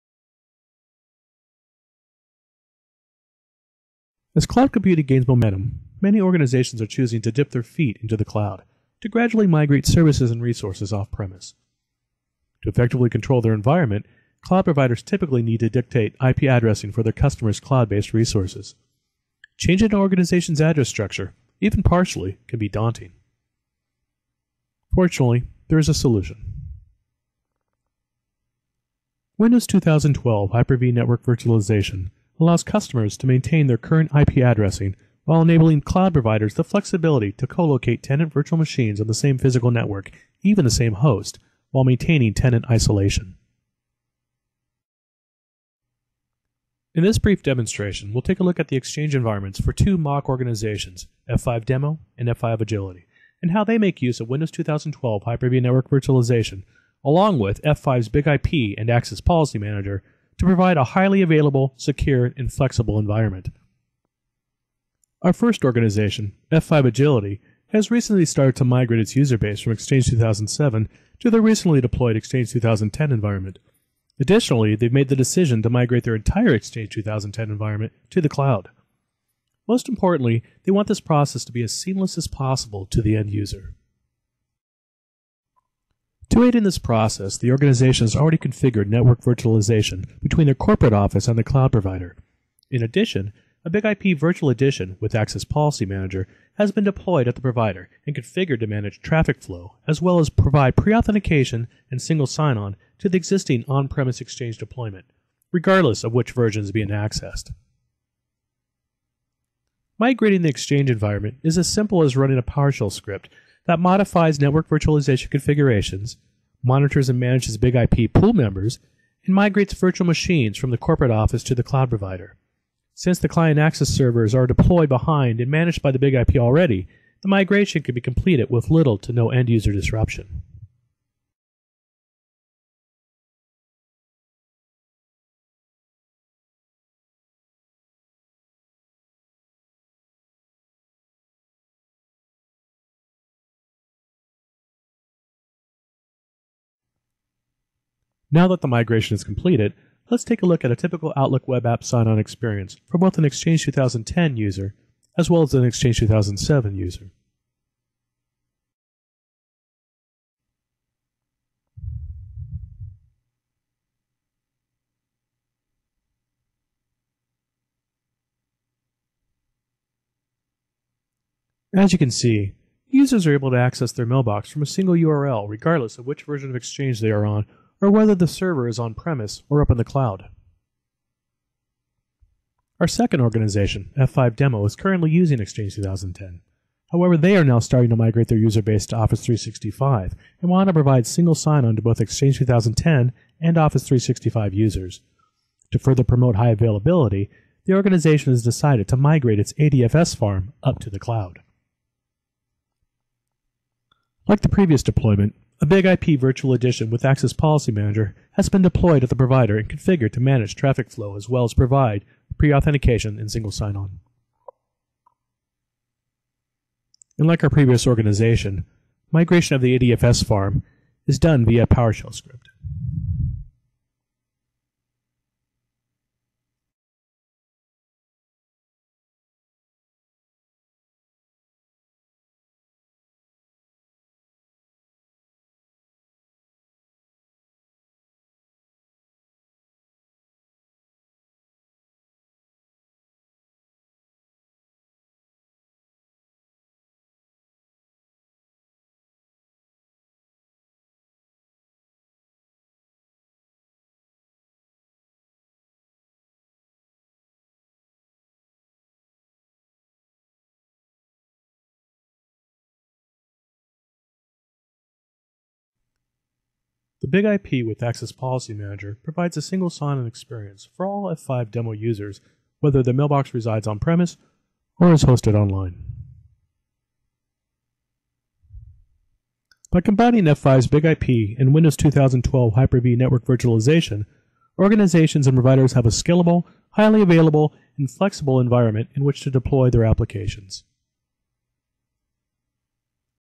F5 Big-IP and Hyper-V Network Virtualization
Overview of BIG-IP APM and Microsoft's Windows 2012 Hyper-V Network Virtualization.